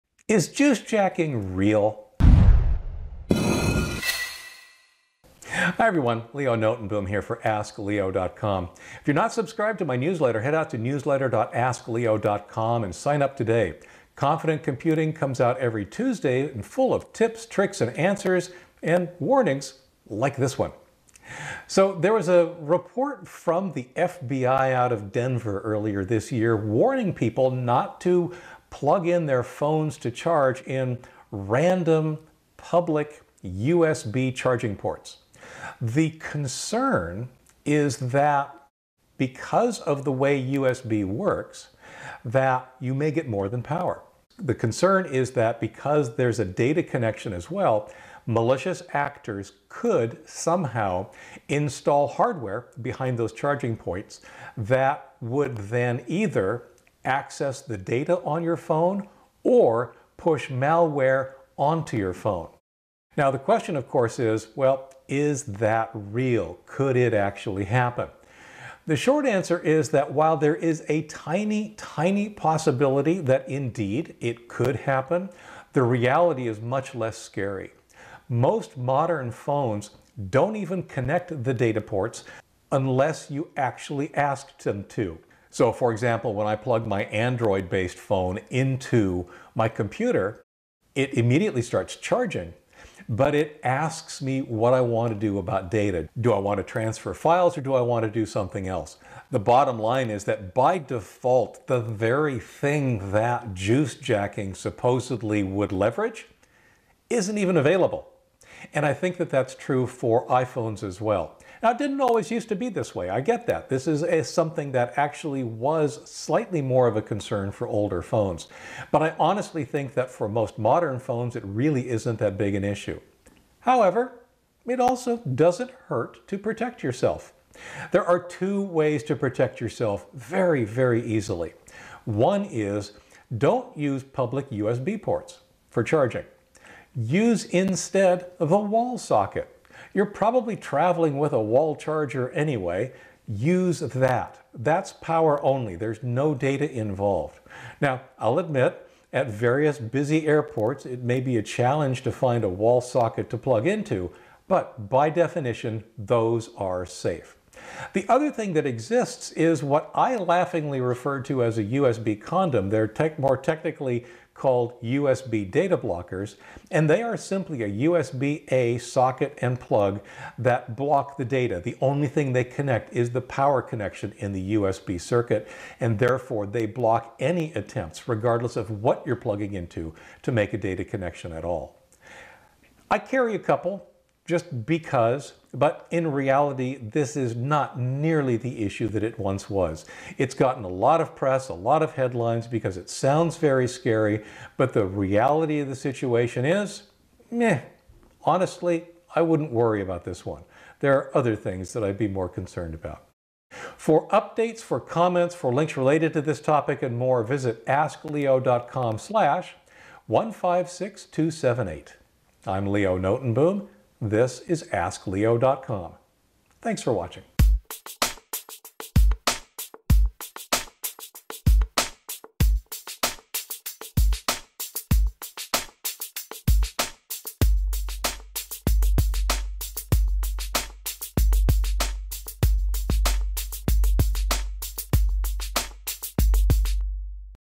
Is “Juice Jacking” Real?
⚠️ The FBI released a warning against "juice jacking", but is it really something you need to worry about?

⚠️ Juice jacking
While theoretically possible, “juice jacking”, or the malicious transfer of data while connected to an unknown USB charging port, is both rare and easily thwarted. Most current mobile devices won’t even allow data connections by default, and using a wall charger or USB data blocker also neutralizes the threat.

Chapters
0:00 Juice Hacking
0:40 The warning
1:10 The tech
2:00 Protect yourself